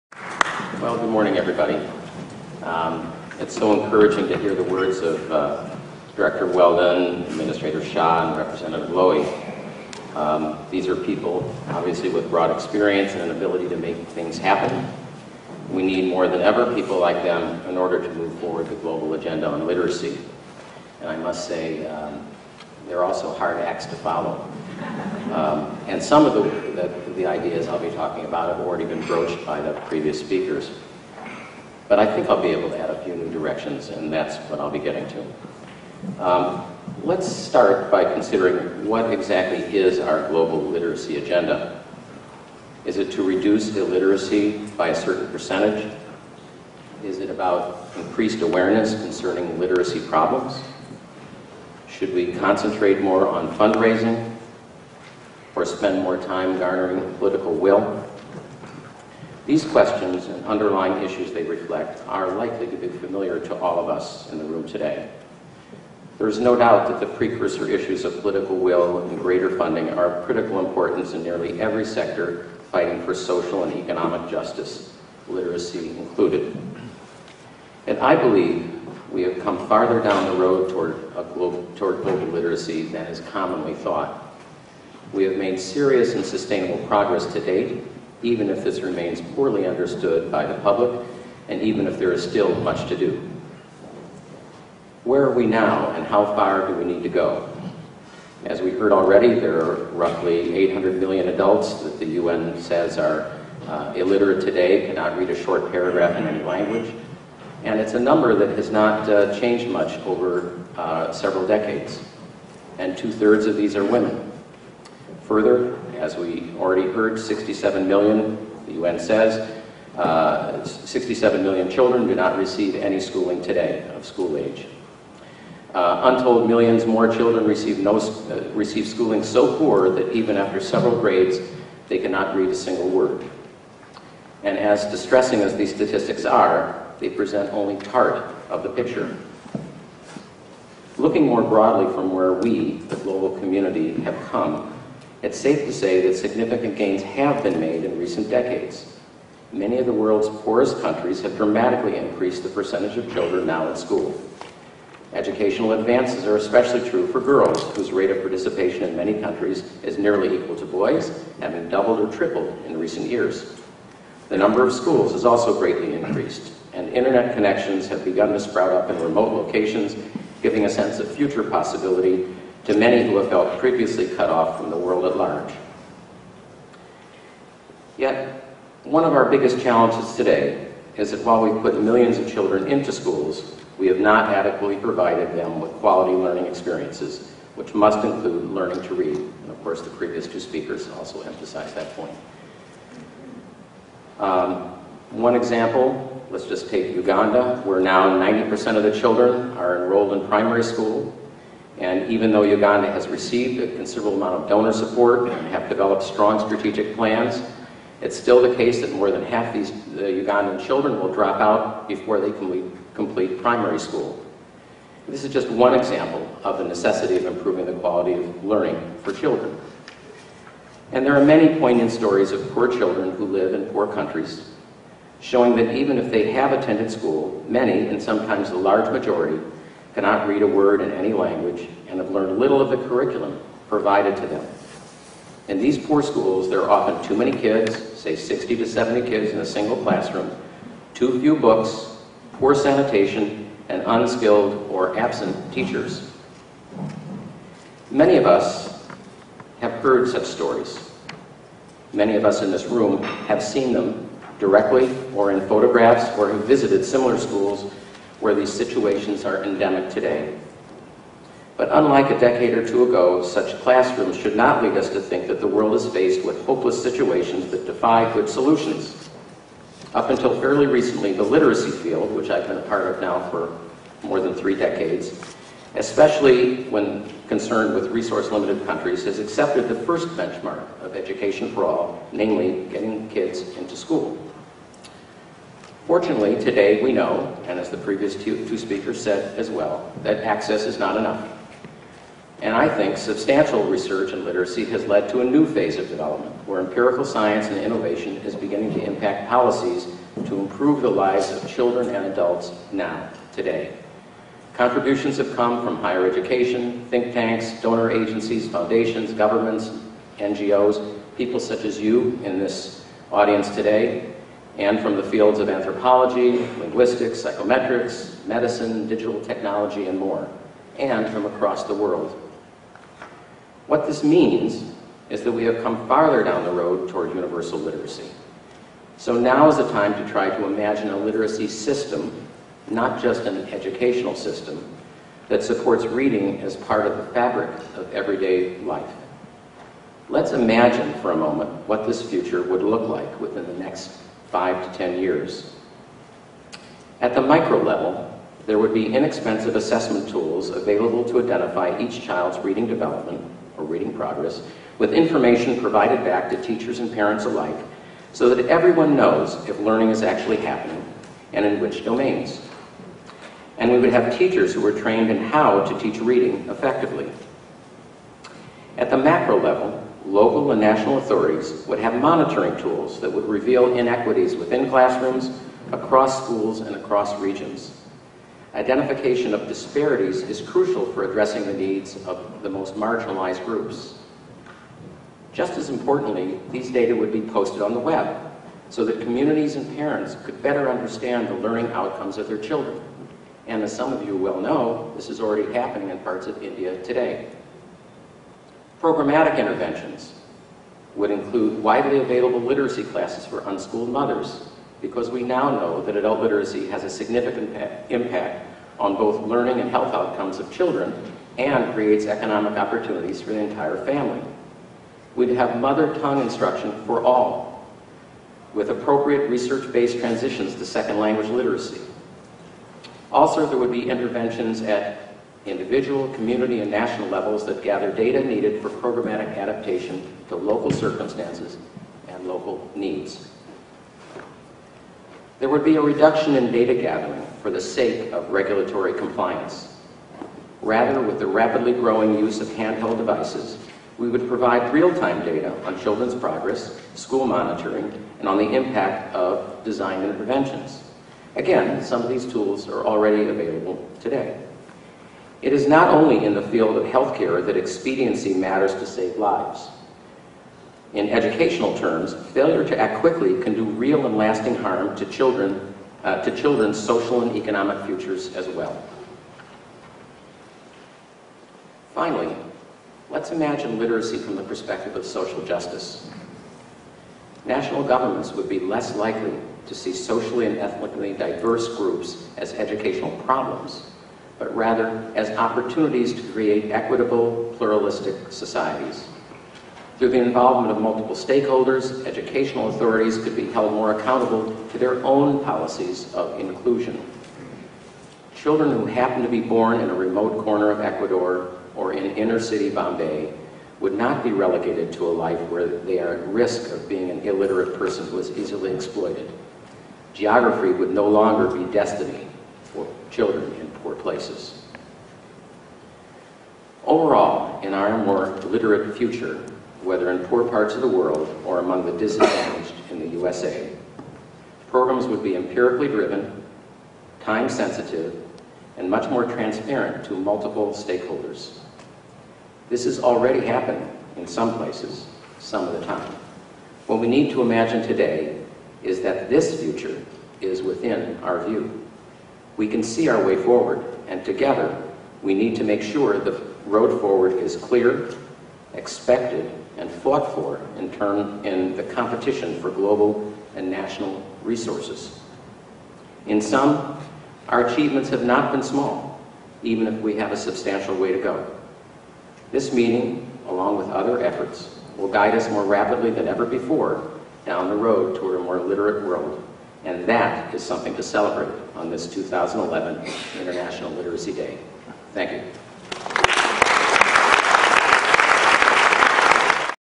Addressing Literacy Issues: Dan Wagner
The following is a video archive of All Children Reading: An International Literacy Day Event.